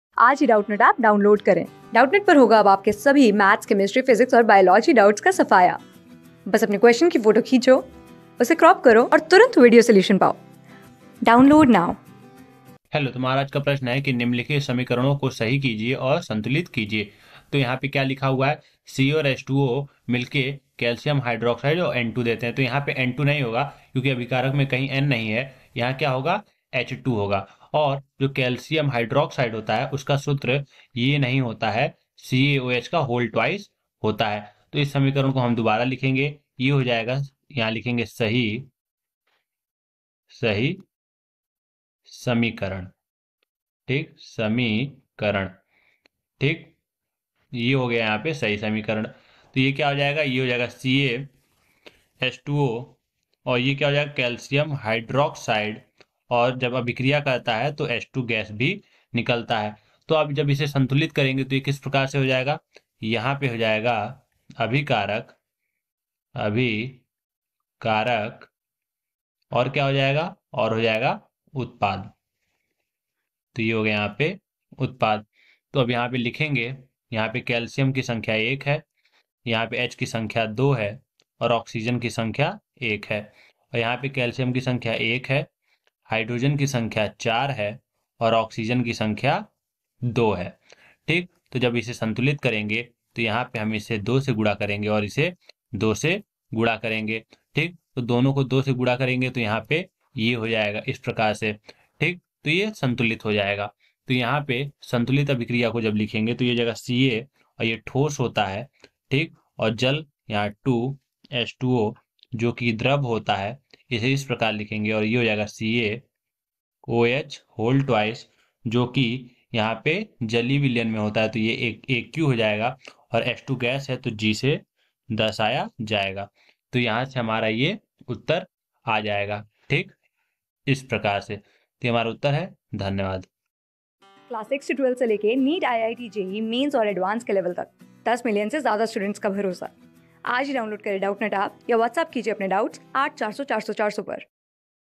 निम्नलिखित समीकरणों को सही कीजिए और संतुलित कीजिए : Ca + H_2 O to CaOH + N_2 | CLASS 10 | रासायन...
निम्नलिखित समीकरणों को सही कीजिए और संतुलित कीजिए : Ca + H_2 O to CaOH + N_2
Class: 10
Subject: CHEMISTRY
Chapter: रासायनिक अभिक्रियाएँ तथा अभिकरणों
Board:FOUNDATION

👉 Have Any Query? Ask Us.